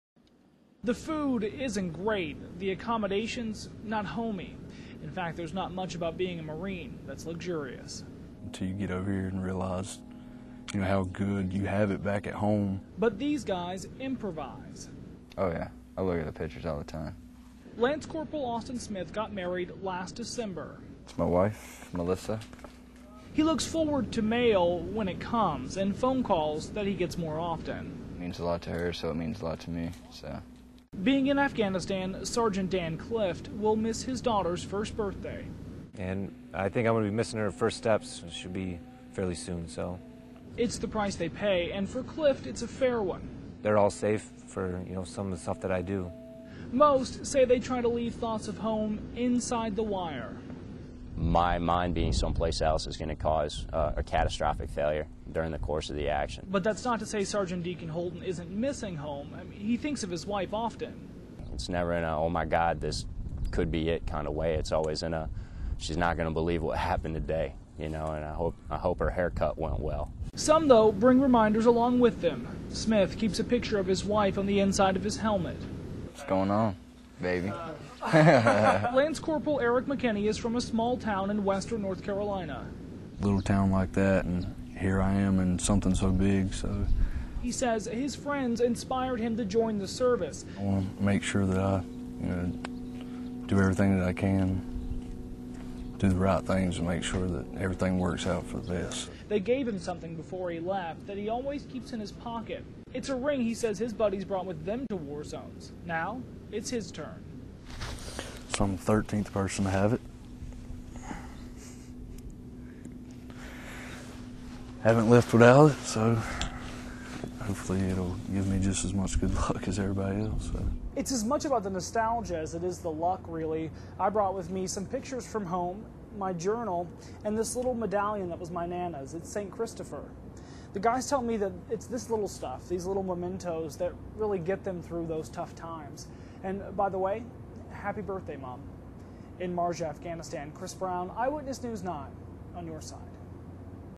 From The Frontline: Momentos From Home
Just because they're 7,000 miles away doesn't mean our Marines in Afghanistan aren't thinking of their family and friends.

From the frontline, Chris Brown shows us how creature comforts give some of our Camp Lejeune Marines peace and, they hope, a little luck too.